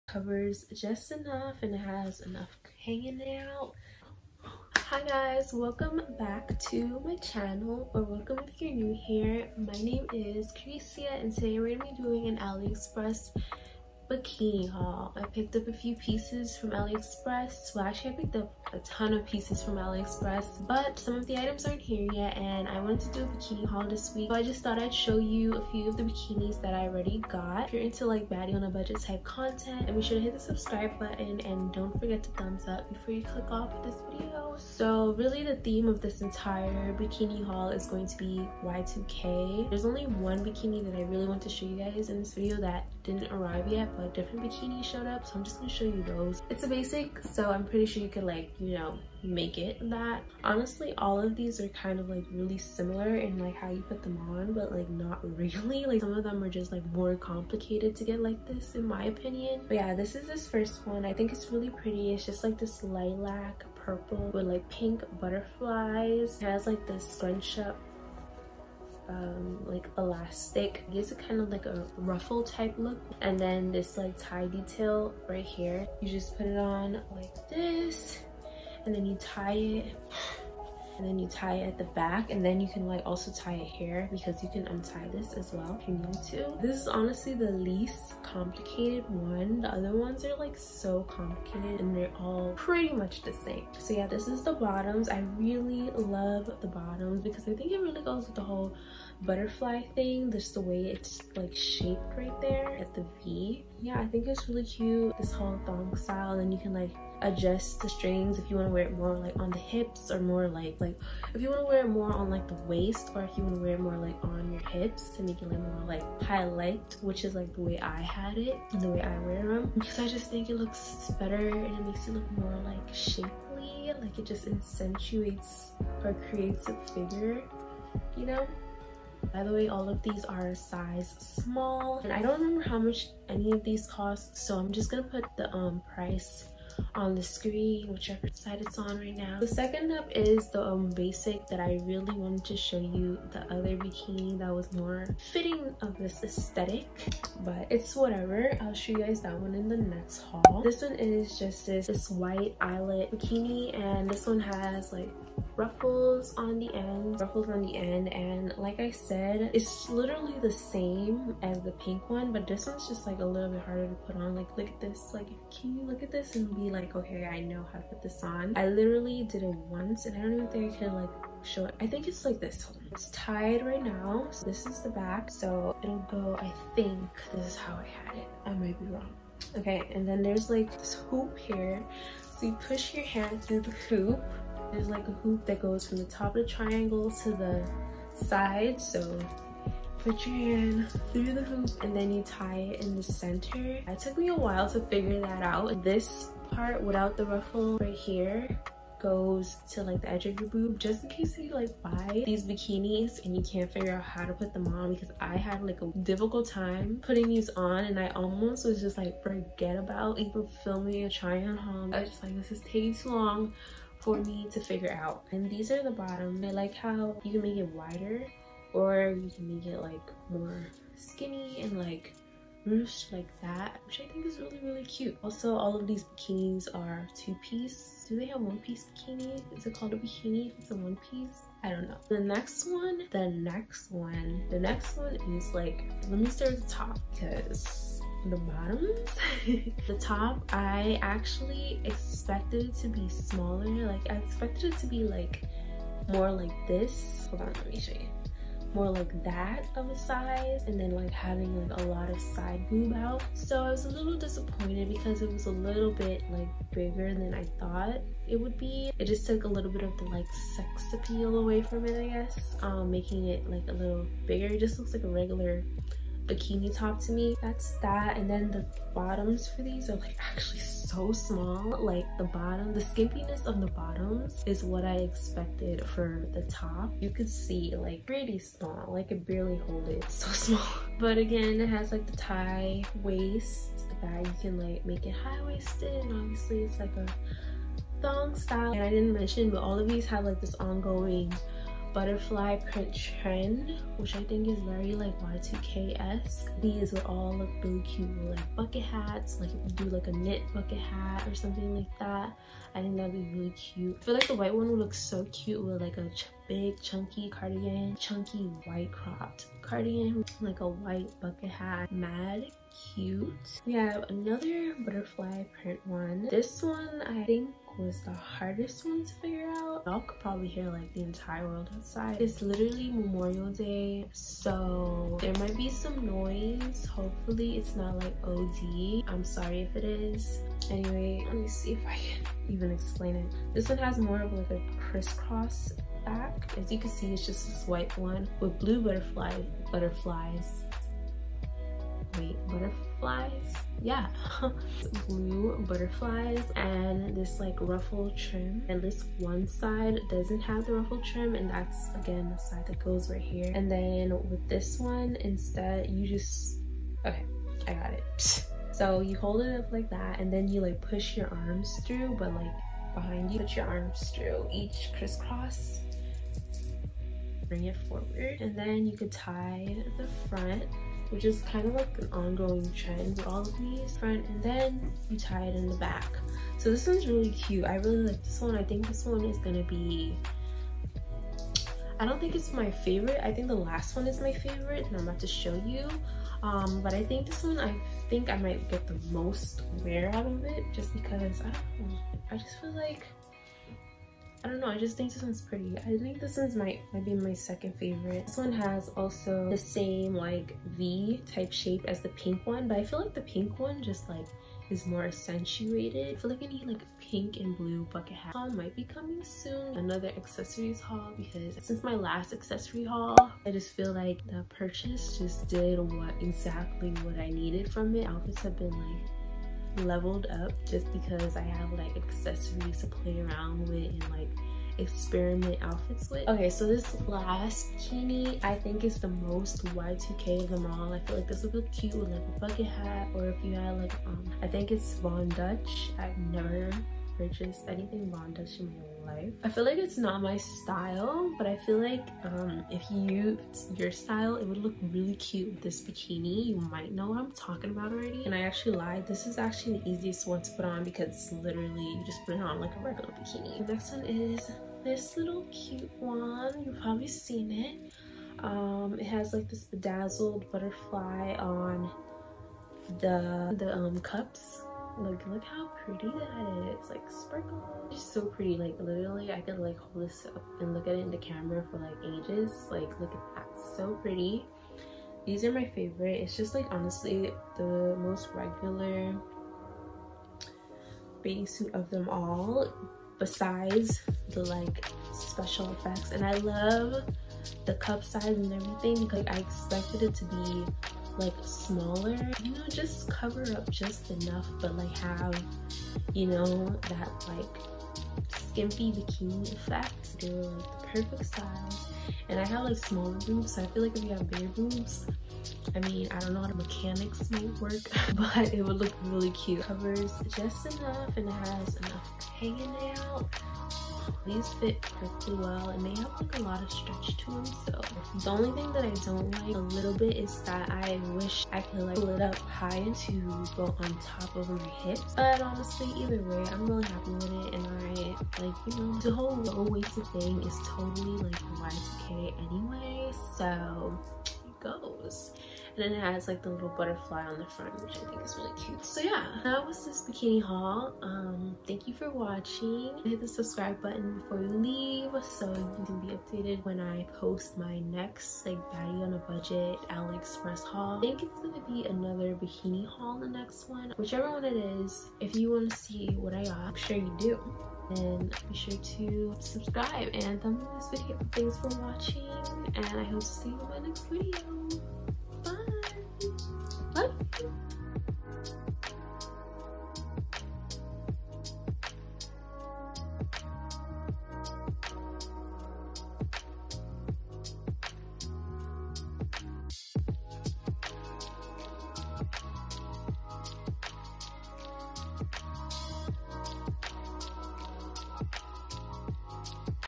ALIEXPRESS TRY ON BIKINI HAUL | Baddie on a Budget | Size Small
ALIEXPRESS TRY ON BIKINI HAUL | Baddie on a Budget

Another baddie on a budget and swimwear/swimsuit haul! both are long overdue!

Links:
Bikini 1

Bikini 2

Bikini 3

Bikini 4

Bikini 5